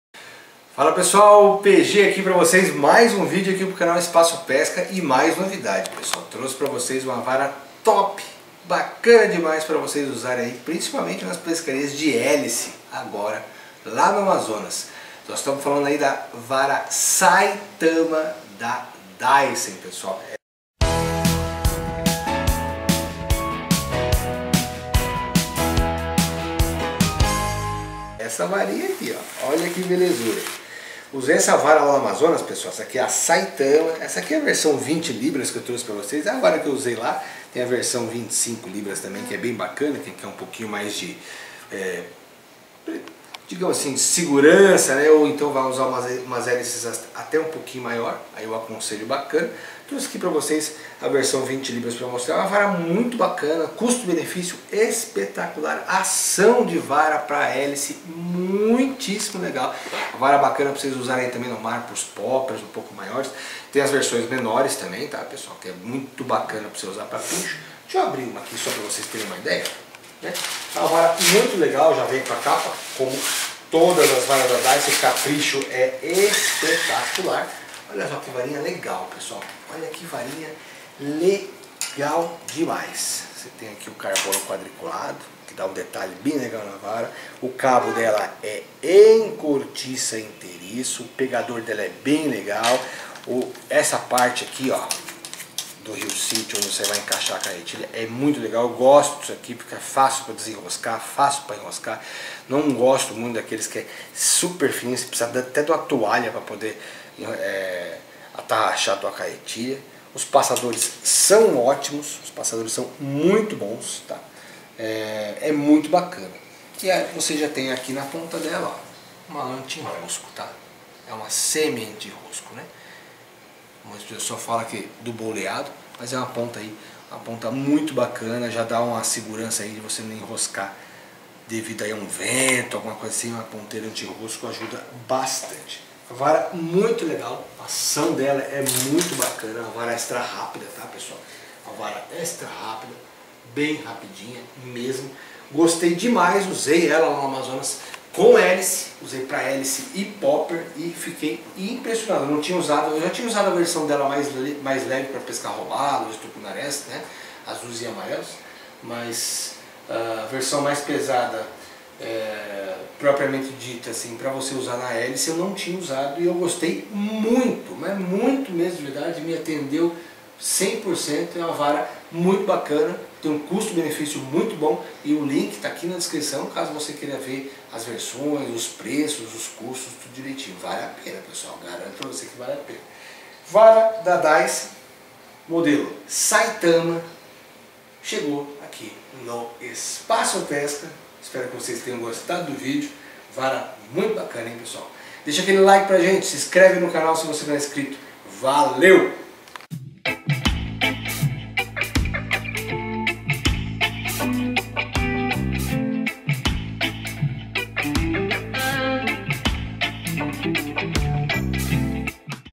Vara Saitama da Daisen - Novidade aqui no Espaço Pesca.
Chegou aqui no Espaço Pesca a vara Saitama da Daisen.
Vara muito top, a mesma que eu usei nas pescarias no Amazonas para iscas de Hélice.
Vara bem forte, ótimo acabamento e muito bom custo benefício.
Abraços
P.G
Segue abaixo o links dda vara Saitama, sóclicar para ver mais e poder também adquirir.

▬▬▬▬▬▬ Contatos profissionais  ▬▬▬▬▬▬▬
- Parcerias